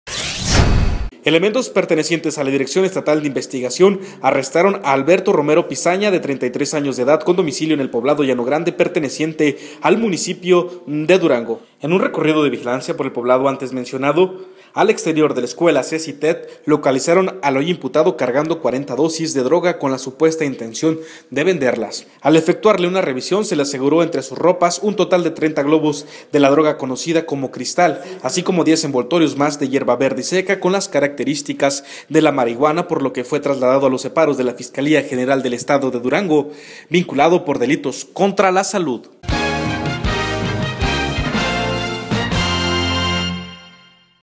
Le cachan mota y cristal en Llano Grande
Los elementos pertenecientes a la Dirección Estatal de Investigación (DEI), arrestaron a Alberto Romero Pizaña de 33 años de edad, con domicilio en el poblado Llano Grande perteneciente al  municipio de Durango, en recorrido de vigilancia, ésto al exterior de la escuela CECyTED, donde localizaron al hoy imputado cargando 40 dosis de droga, para su venta.